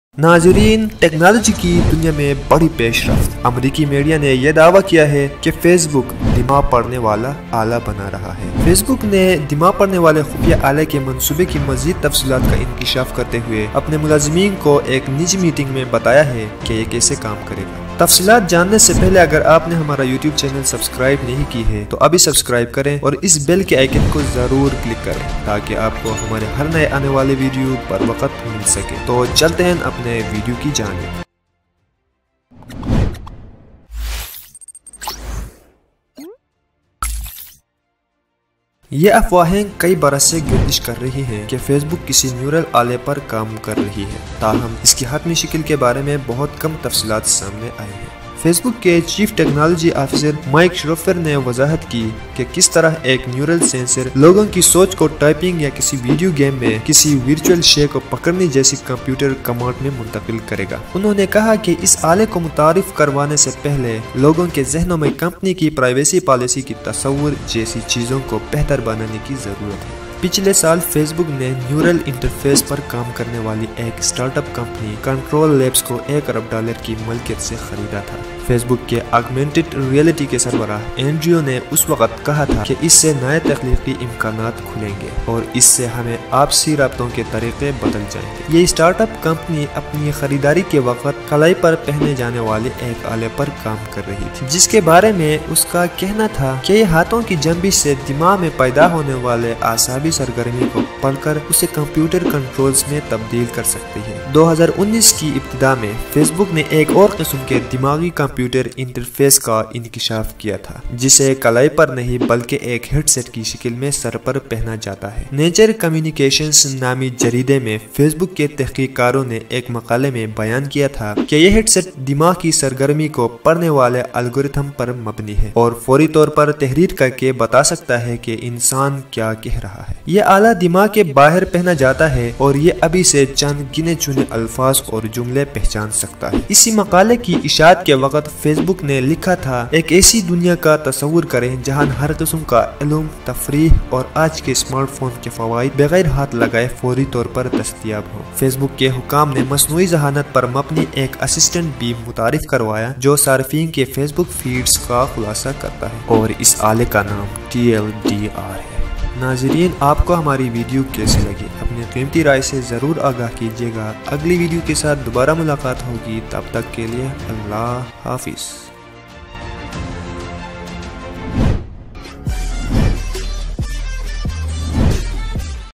Facebook reveals plans for mind-reading device in leaked audio recording
Facebook has revealed more details about its secretive plans to release a mind-reading device, telling employees in a private meeting on Tuesday how it will work.
In an audio recording of the meeting, obtained by BuzzFeed News, Facebook’s chief technology officer Mike Schroepfer explained how a neural sensor would translate people’s thoughts into computer commands, such as typing or holding a virtual object in a video game.
“We all get the privilege of seeing the future because we are making it,” he said.
Mr Schroepfer also said the firm’s public perception when it came to issues like privacy would need to be addressed before releasing any such product.
“We have to build responsibly to earn trust and the right to continue to grow. It’s imperative that we get this right so that people around the world get all these amazing technologies… without experiencing the downsides."
Rumours that the technology giant has been working on a neural device have been circulating for several years, though few details have emerged of what form it will take.
Last year, Facebook bought a neural interface startup called CTRL-Labs in a deal estimated to be worth around $1 billion (£740 million). Facebook’s head of augmented reality, Andrew Bosworth, said at the time that the acquisition would “open up new creative possibilities” and “change the way we connect”.
The US-based startup was working on a high-tech wristband at the time of the takeover, which it claimed could measure neural activity from physical gestures and translate it into computer controls.
Earlier in 2019, Facebook divulged details of a separate type of brain-computer interface that used a headset instead of a wristband to read thoughts.
In a paper published in Nature Communications, Facebook researchers described how the headset relied on an algorithm capable of decoding brain activity to instantly transcribe what a person is saying into text.
The non-invasive wearable device could already recognise a small set of words and phrases.
In a blog post published at the same time as the research, Facebook wrote: “Imagine a world where all the knowledge, fun, and utility of today’s smartphones were instantly accessible and completely hands-free.”
a computer mouse: A headset used by Facebook’s brain-machine interface researchersFacebook/ Nature Communications© Provided by The Independent A headset used by Facebook’s brain-machine interface researchersFacebook/ Nature Communications
During the staff meeting on Tuesday, executives also unveiled an AI assistant tool that summarizes news articles on users’ Facebook feeds.
The TLDR tool - named after the acronym for “too long, didn’t read” - will condense long form articles to bullet points, potentially depriving even more valuable web traffic away from news publishers.
Facebook did not immediately respond to a request for comment from The Independent.